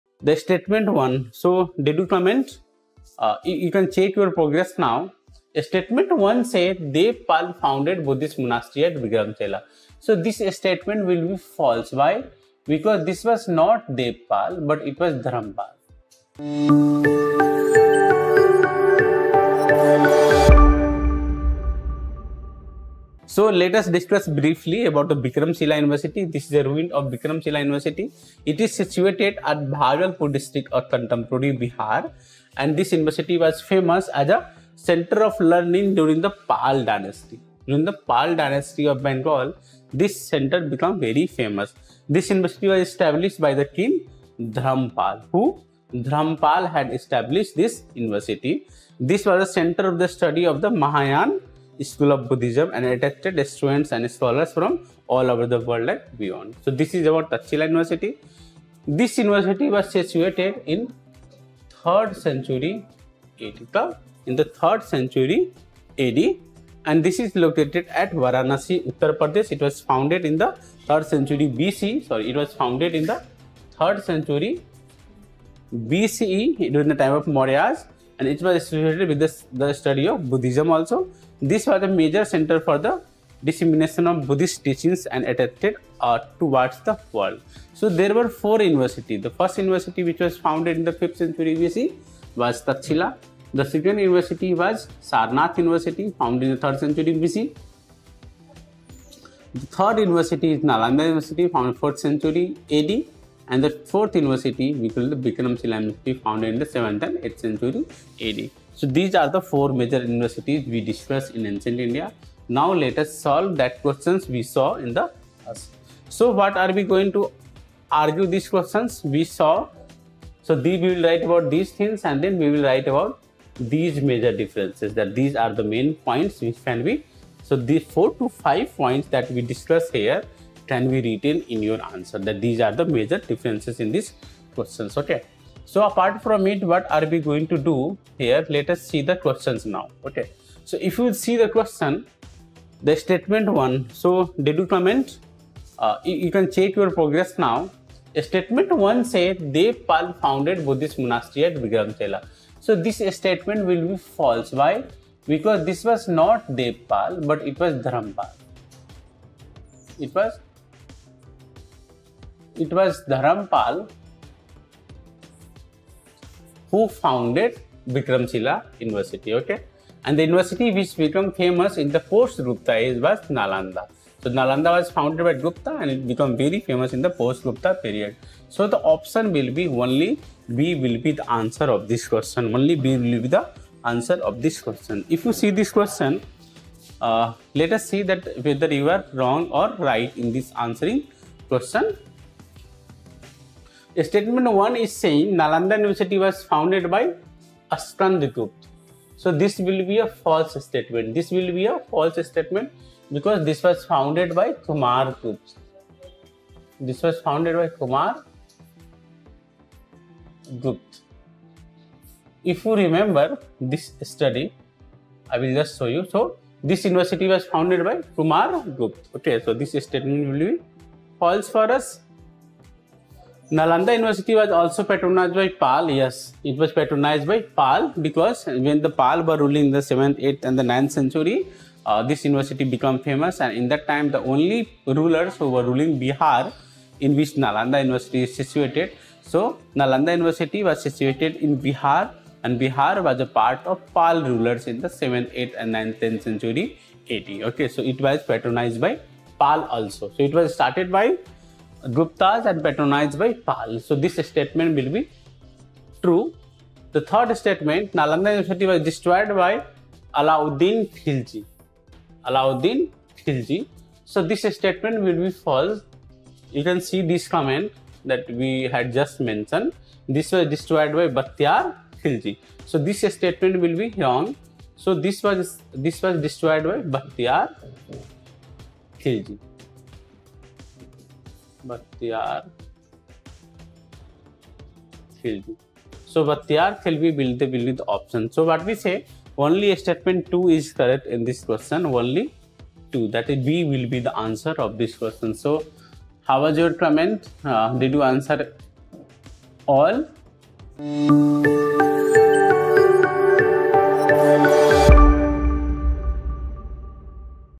Ancient Universities of India | History Optional for UPSC | Plutus IAS
Explore the rich heritage of Ancient Universities in India with Plutus IAS! In this enlightening video, we delve into the historical significance and contributions of India's ancient centers of learning, crucial for History Optional in UPSC. From Nalanda to Takshashila, discover the intellectual hubs that shaped India's cultural and educational landscape.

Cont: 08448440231